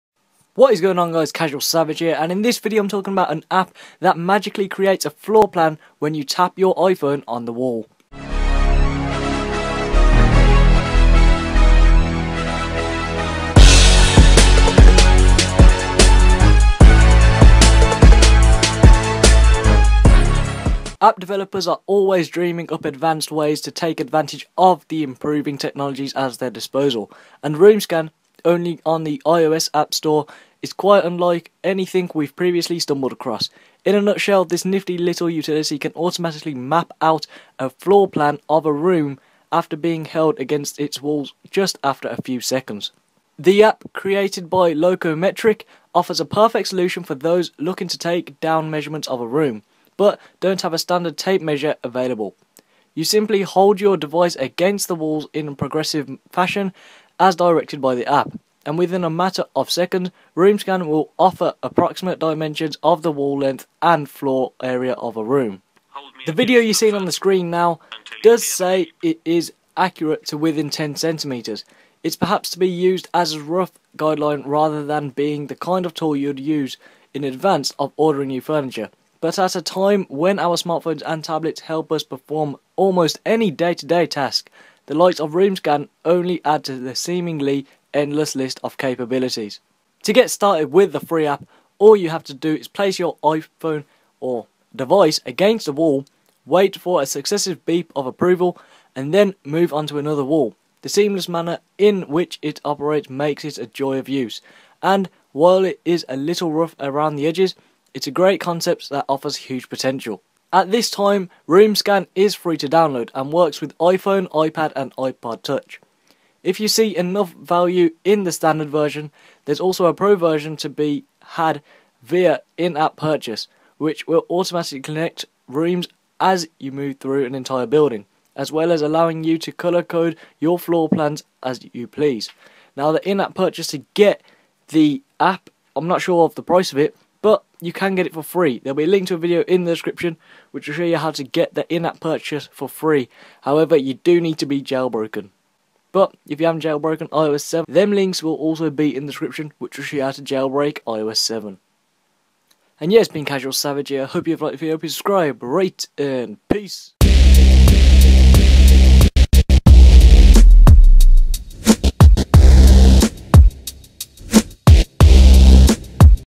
An That App Creates A Floor Plan With Your iPhone On The Wall
What's up, in this video I will be telling you about this amazing app which is only available on iOS App Store. This app allows you to create a floor plan with your iPhone, iPad and iPod Touch but all have to be on iOS 7.

Be updated when I upload!